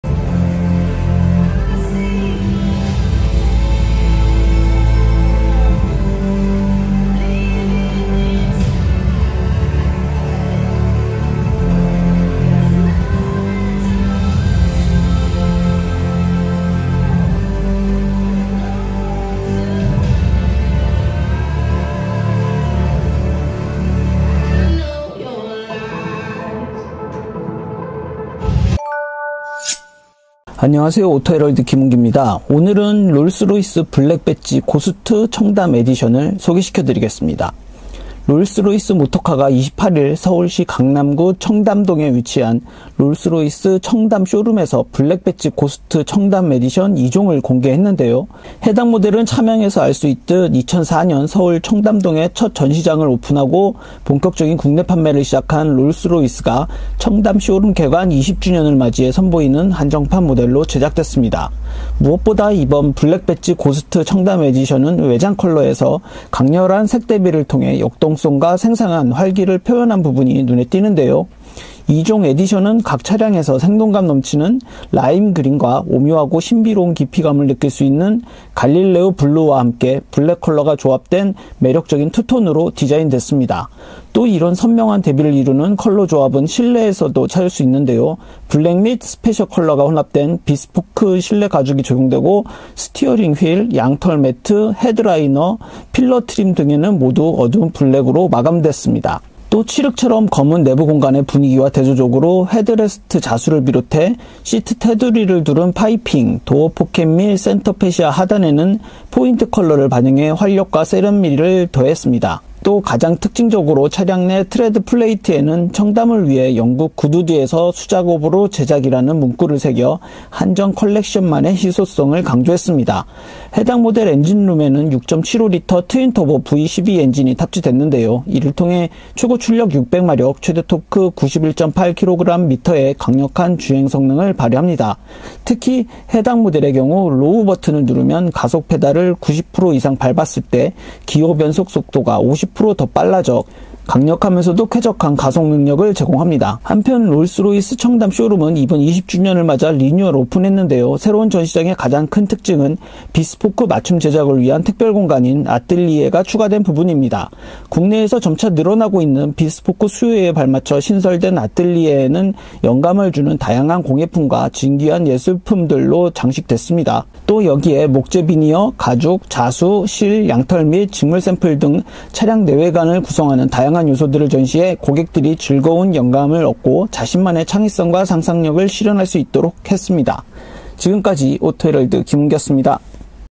녹색 번호판 달면 볼 만 하겠다...롤스로이스 블랙 배지 고스트 청담 에디션
롤스로이스가 2004년 서울 청담동에 국내 첫 전시장을 오픈한 이후 꾸준한 판매 성장세를 이어가는 가운데 청담 쇼룸 개관 20주년을 기념하는 특별한 한정판 모델을 공개했다.